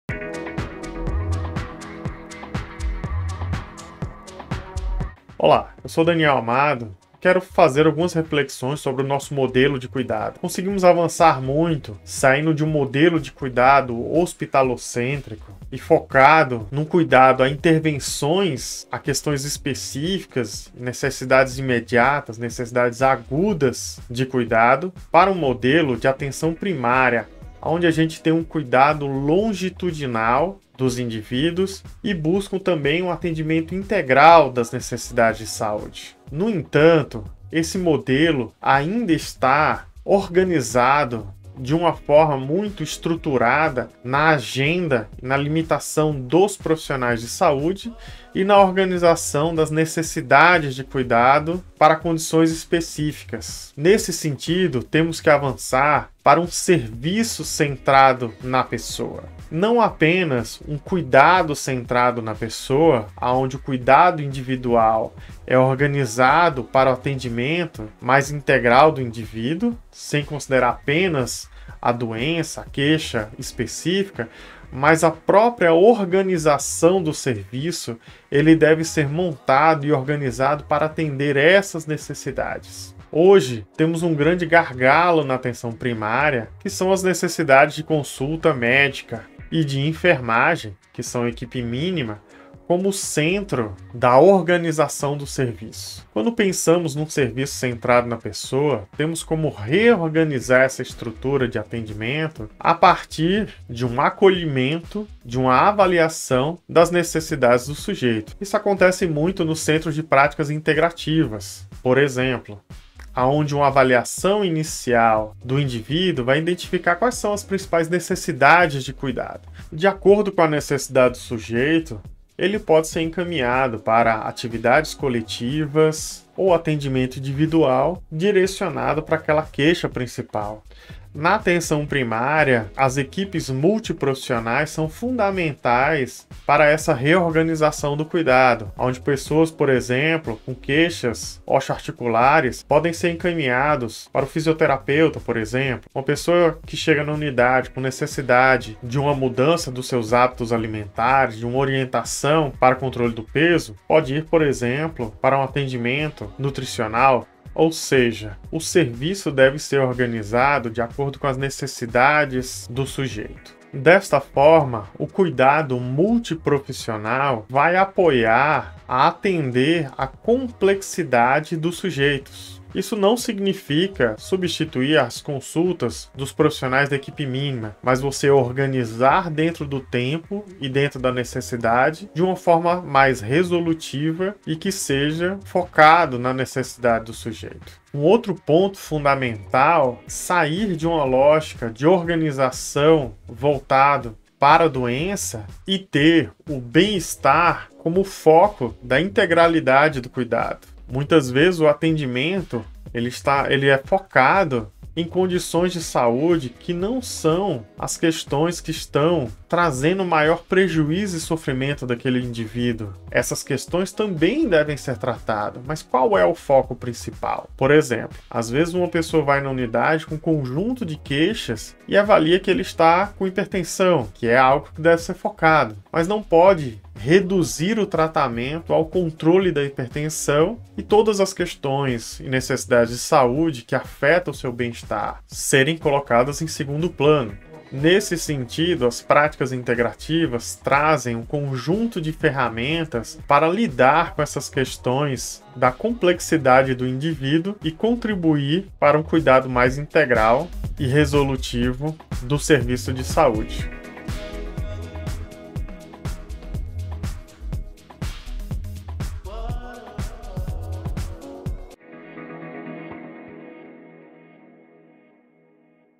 Serviço Centrado na Pessoa e Práticas Integrativas
Além do atendimento em saúde, que deve ser centrado na pessoa. O serviço de saúde também deve se organizar de forma a atender o sujeito e sua complexidade, não devemos moldar as necessidades dos sujeitos a conveniência e estrutura rígida dos serviços de saúde. Experiências muito interessantes tem ocorrido nos Centros de Práticas Integrativas e Complementares em Saúde.

Tem gostado do projeto e quer ajudar? conheça as formas de apoiar a continuidade e crescimento desse projeto